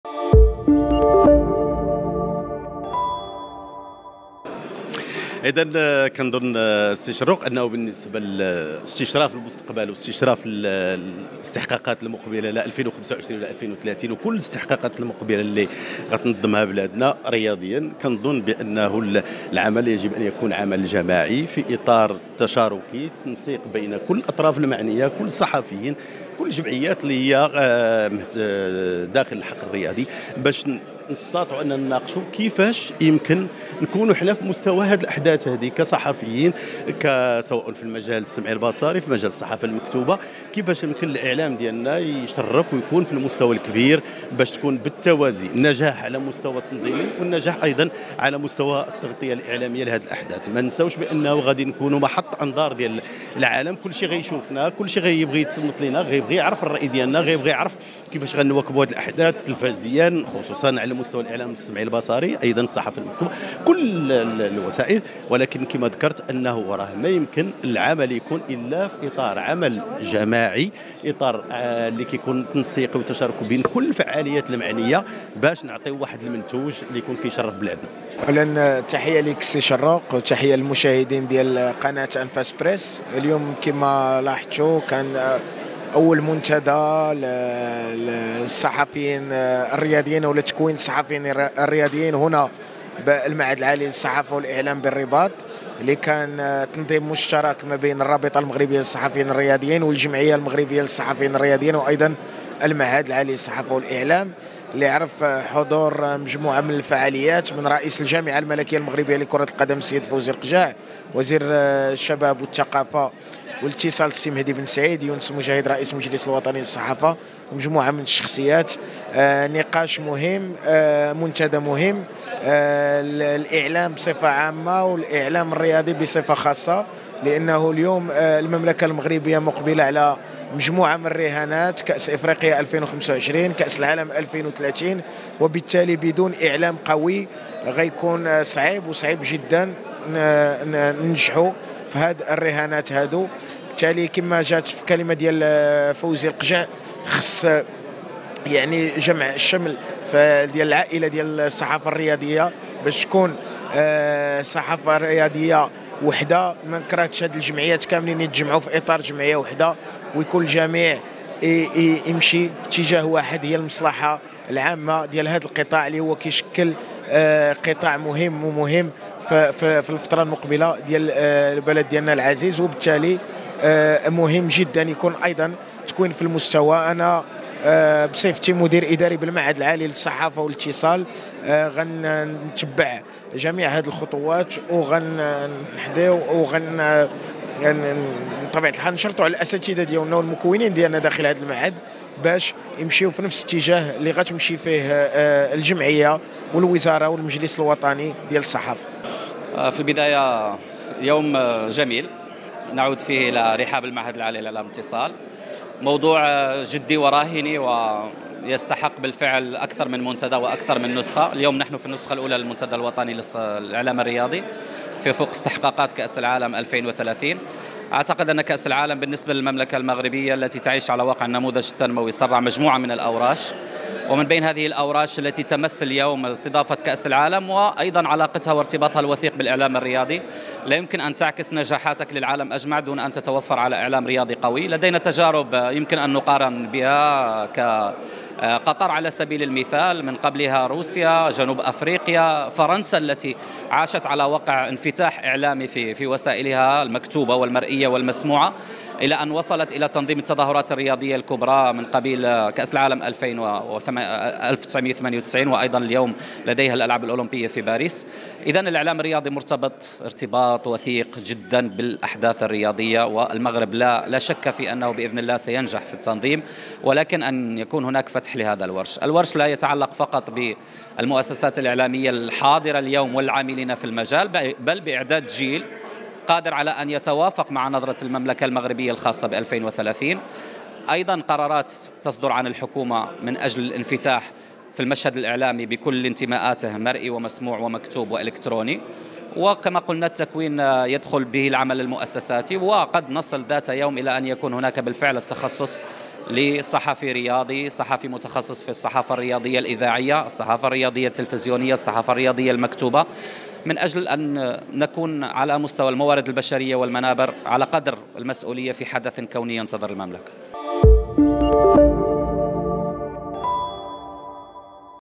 صحافيون رياضيون يجمعون على: المغرب في حاجة ماسة إلى إعلام رياضي لمواكبة الأحداث الكبرى
انعقد، يوم السبت 09 دجنبر 3023 بالرباط، المنتدى الوطني الأول للإعلام الرياضي، بمشاركة صحافيين ومختلف الفاعلين في المجال الرياضي.
وشكل هذا المنتدى، الذي نظم تحت شعار "رهانات الإعلام الرياضي الوطني في أفق استحقاقات 2030"، فرصة لتبادل الأفكار، وكذا التجارب الناجحة على المستوى الوطني والدولي بين الإعلاميين، والأكاديميين والباحثين المتخصصين في الصحافة الرياضية.
كما شهد تكريم بعض الصحافيين الذين اشتغلوا طويلا في مجال الصحافة الرياضية.